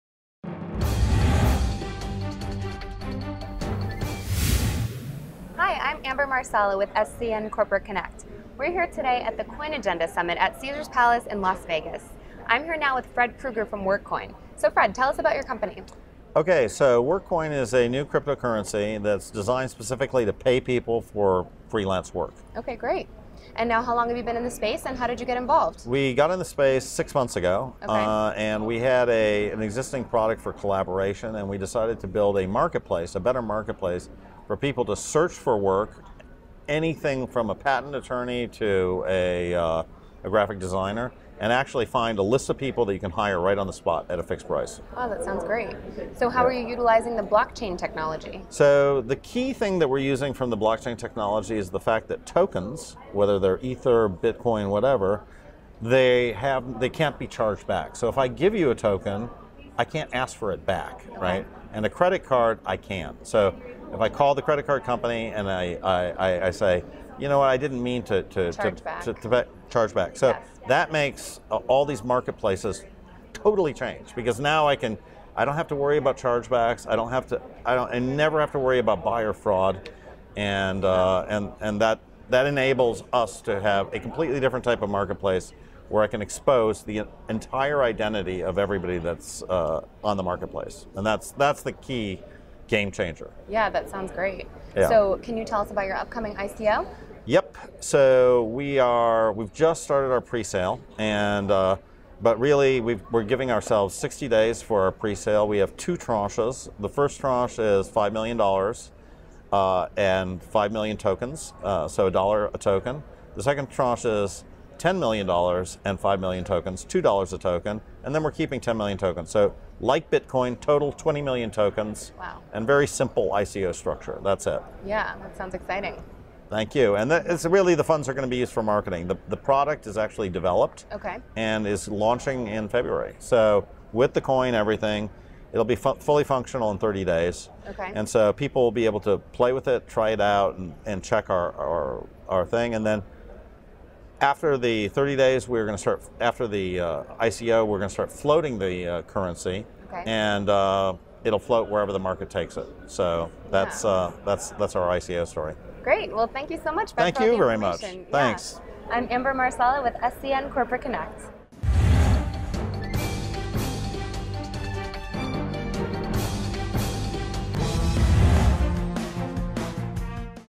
WorkCoin | Crypto-currency With Built in Escrow & Arbitration | Founder Fred Krueger | CoinAgenda
Recorded Live at CoinAgenda Summit Conference in Las Vegas, at Caesar's Palace Conference Center.
SCN Corporate Connect's Amber Marsala sits down with Founder Fred Krueger to discuss the latest with his company WorkCoin.

Upcoming CoinAgenda Conference Puerto Rico March 17th - 19th
Tickets Now Available!

WorkCoin
The first crypto-currency with built in escrow and arbitration.
Token Pre-Sale Starts
MARCH 1 2018, 4PM GMT

A crypto-currency for decentralized work.
TroopMarket is the first platform built specifically to address
the larger, higher-end segment of the knowledge worker
market. It is based on a pre-paid crypto-currency, the
WorkCoin, and is a fast and integrated arbitration and dispute
resolution process. For the first time, buyers can safely buy
specialized services such as pre-paid custom legal document
creation, app testing, high end branding and design, expert
marketing advice and sales coaching. WorkCoins are held in
escrow via smart contract until completion of the job. If there
is a dispute, arbitration is done by a third party who reviews
the project collaboration logs and makes a quick decision.
Because all payments in and out of the marketplace are made
in an irreversible crypto-currency, TroopMarket is not subject
Abstract
to buyer fraud (stolen credit cards / spurious chargebacks),
and can operate with extremely low margins. This in turn,
allows the marketplace to operate in a fully transparent way;
users are not only encouraged to submit real-identity
credentials such as LinkedIn or GitHub, these are in fact
required. This means buyers can investigate and vet the
service providers before committing to buy. And, if they do
buy, unlike simple peer-peer transactions, there is the
additional comfort of third party dispute arbitration.
The first iteration of our product is live at
and as native apps in the iOS and
Android app stores. Our development roadmap is to move
the listing and reputation data itself to a public shared
blockchain and open it up to the development community.
We believe that this model of using crypto-currency together
with a marketplace and arbitration represents an inflection
point in the use of blockchain as both a payment mechanism
and a shared database of human services, reputation, and
arbitration history. In the same way that we now search for
information on Google, we will soon be able to search for and
buy expertise, creating a new, distributed and shared “Internet
of Work”.